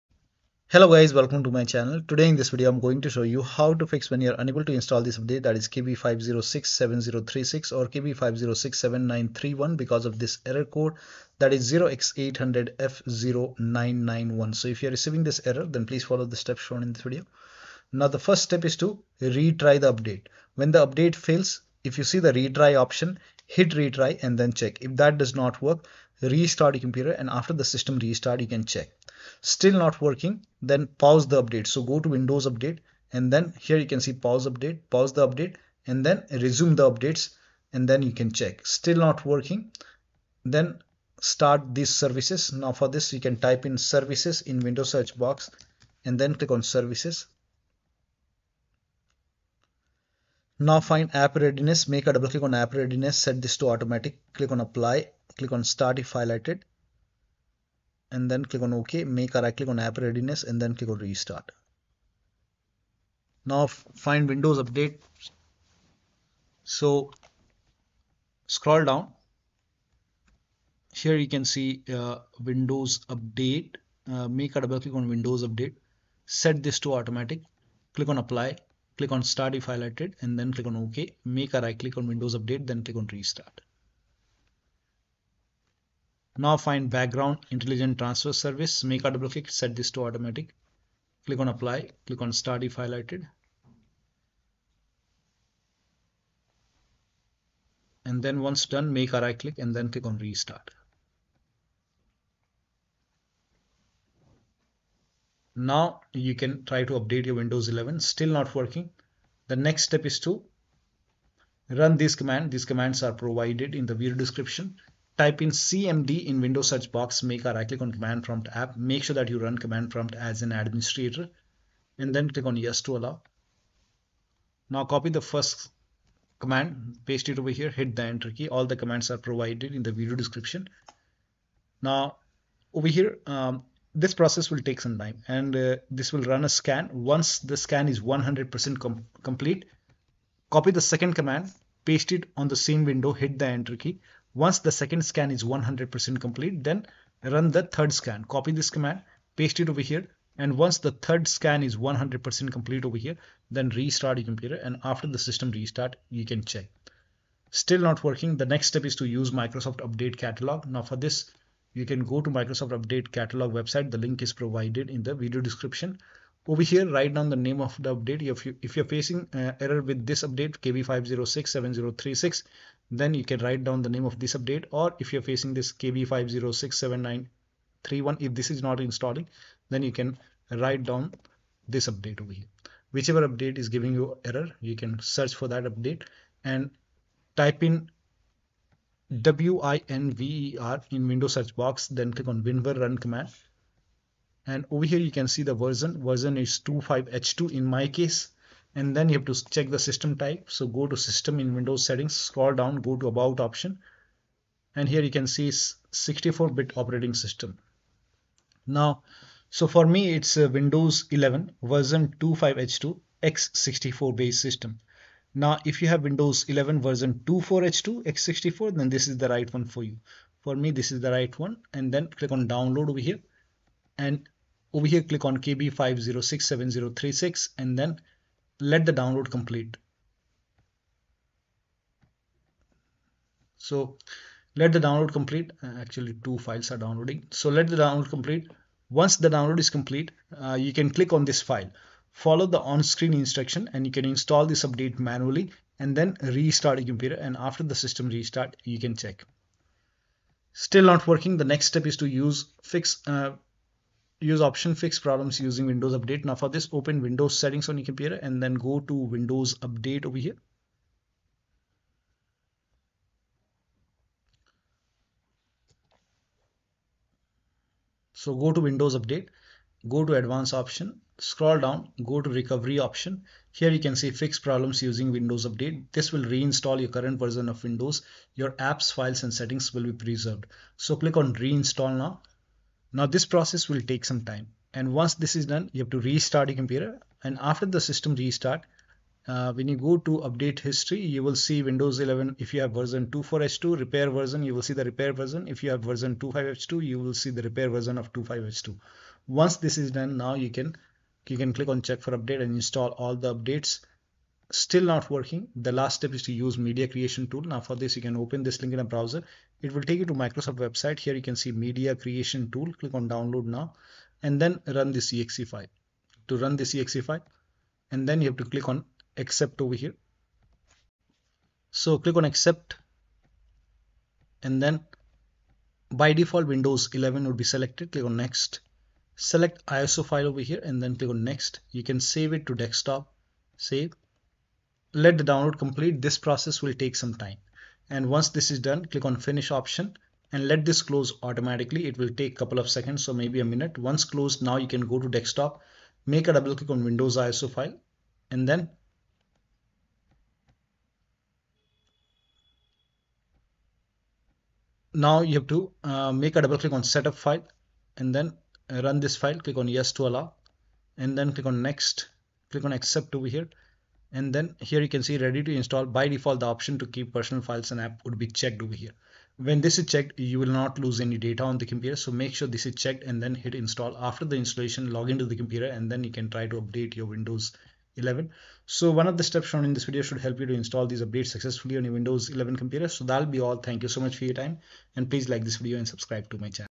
Fix Update KB5067036/KB5067931 Not Installing Install Error 0x800f0991 On Windows 11
Fix Can't Update KB5067036/KB5067931 Install Error Code 0x800f0991 On Windows 11
Retry the update, restart PC
Step 1) Start/Restart App Readiness, Windows Update & Background intelligent transfer service
Step 2) Pause and restart the update
Step 3) Run SFC & DISM Command
sfc /scannow